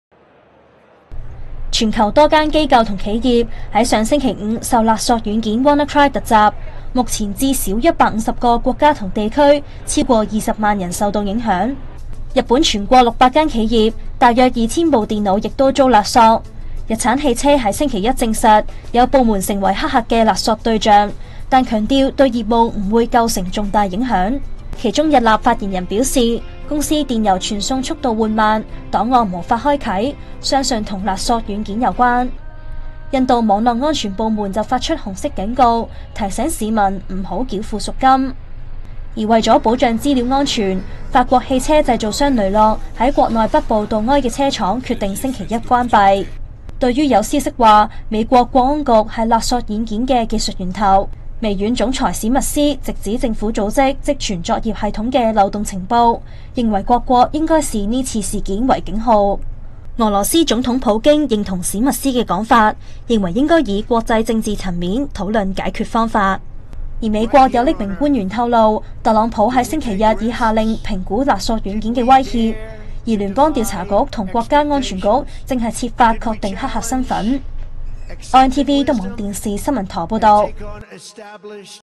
WannaCry蔓延日本 600企業2000電腦中招
神秘黑客近日向全球多間機構及企業，發出勒索軟件「WannaCry」(又名WanaCryptOr 2.0)，索取贖金。日本日產汽車、日立等多家企業亦遭黑客勒索，有電腦保安組織表示，日本全國有600家企業，2000部電腦受影響。

日產汽車周一證實，部分部門成為黑客的勒索對象，但強調對業務不構成重大影響。日立發言人表示，有人向他們索取贖金，該公司的電郵傳送速度緩慢，檔案也無法開啟，故相信事件與勒索軟件有關。日立強調，他們會以安裝軟件解決問題。此外東日本鐵路公司辦公室一部個人電腦亦「中招」，但鐵路服務不受影響。除了企業外，日本亦有個人電腦受影響。

歐洲刑警組織(Europol)發言人溫賴特(Rob Wainwright)周日接受英國電視台訪問時指，今次全球網絡攻擊事件影響超過150個國家地區，逾20萬受害者，當中包括不少大型企業。